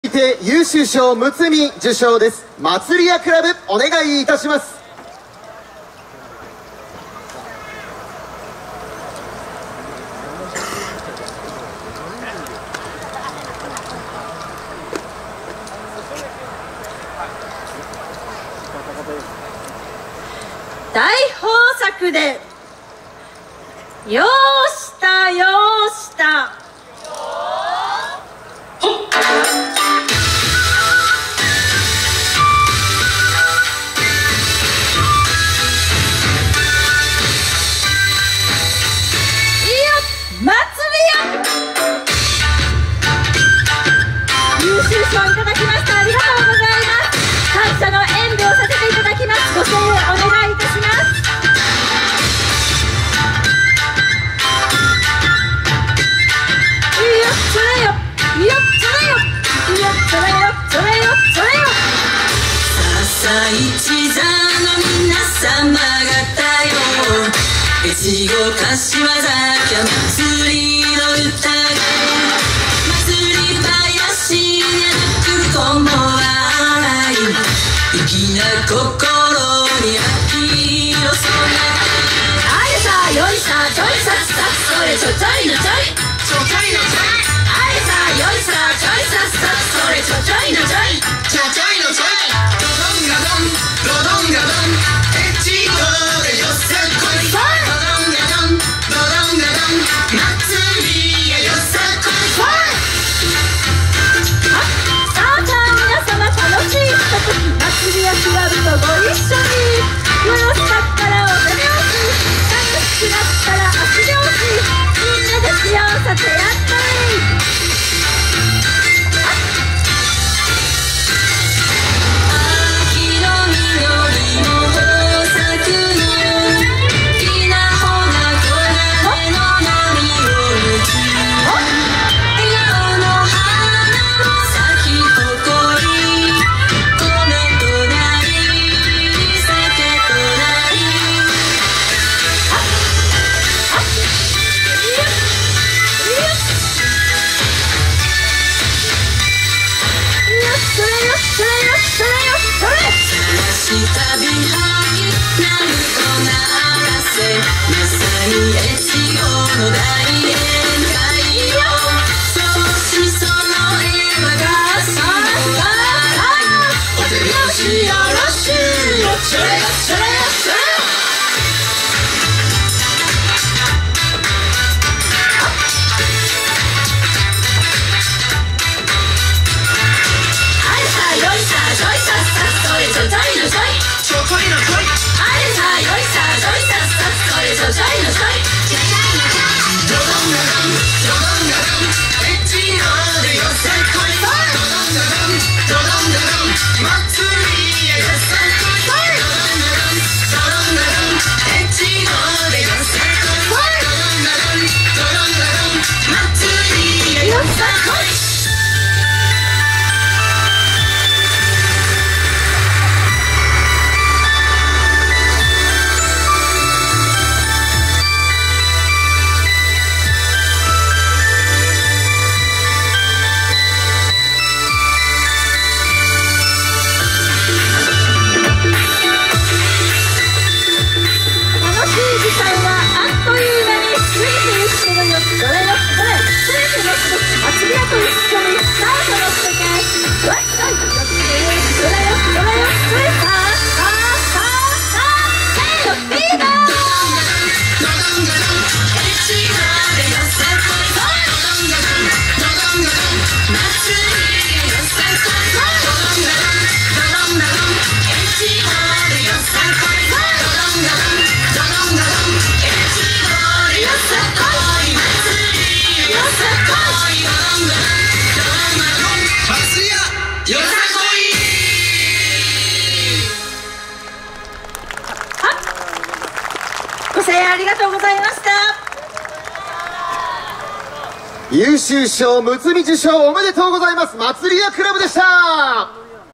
祭や倶楽部 @よさこいとやま(城址公園/受賞演舞) 2018-08-05T19:29
「優秀賞(睦)」受賞おめでとうございます！
2018年8月5日(日)、富山県富山市で開催された「よさこいとやま」(城址公園/受賞演舞)での「祭や倶楽部」さんの演舞です。